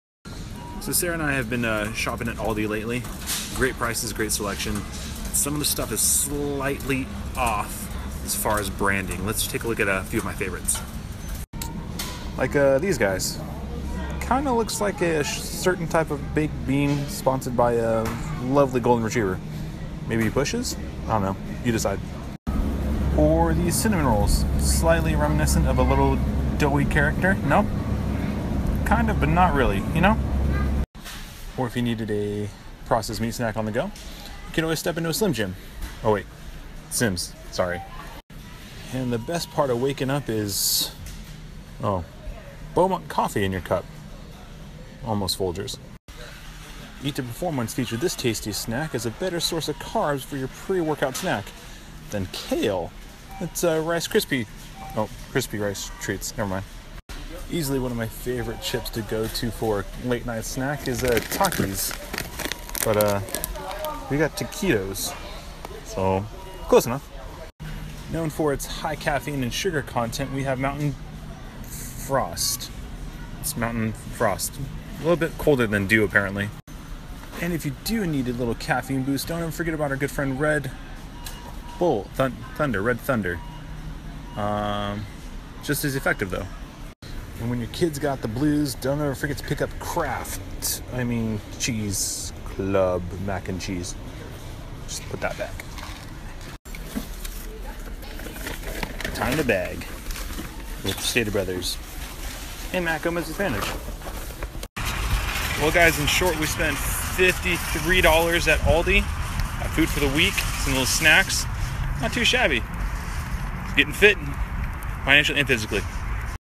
My ACTUAL “First Vlog” (sort of)
Aldi is a great, affordable grocery store! My wife and I love shopping there! But somethings aren't quite what they seem...